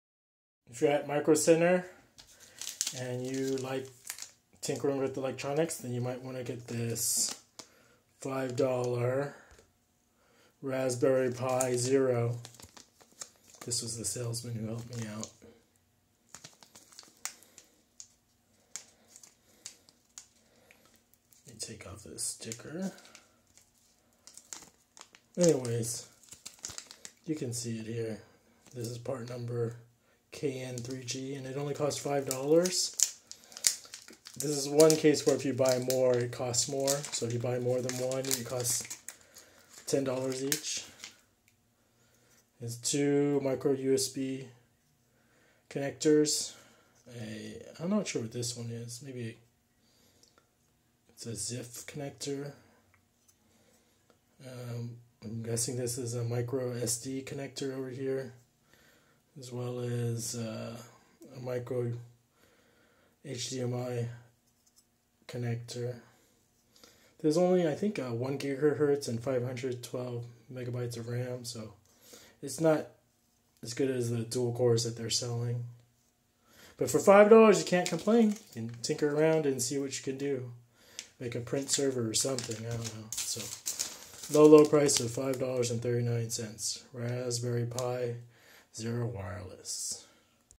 Got a Raspberry Pi Zero W for $5 from microcenter. I don't even know what to do with it?
I've been wanting to try a Raspberry Pi device.  This is one of the cheaper sets you can get which retails for about $10 :Raspberry Pi Zero W

If you're lucky enough, you can grab one at a brick and mortar Microcenter store.  They're on sale for $5.  But if you want more, then you'll have to pay the additional fee of $15 for each.  The alternative is to purchase one.. then leave and come back the next day and buy one more.

REALLY, I don't know what to do with this thing.  I think I need to get a micro HDMI cable to use with this.  That'll probably cost more then the raspberry pi device.

So here's my lame unboxing video